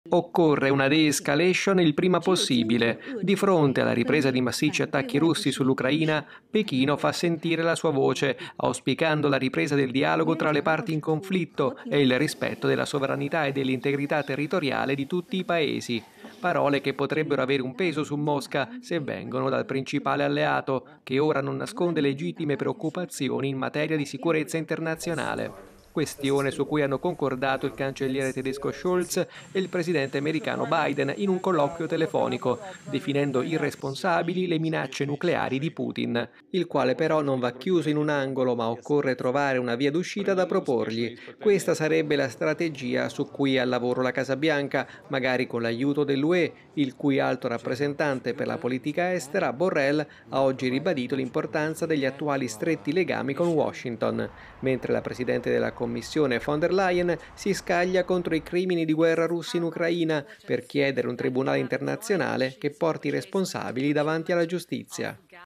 Ucraina, diplomazia lavora allo stop della guerra. In campo anche la Cina
Diplomazia al lavoro per fermare l'escalation di guerra. Scende in campo anche la Cina. Il servizio Pierluigi Vito.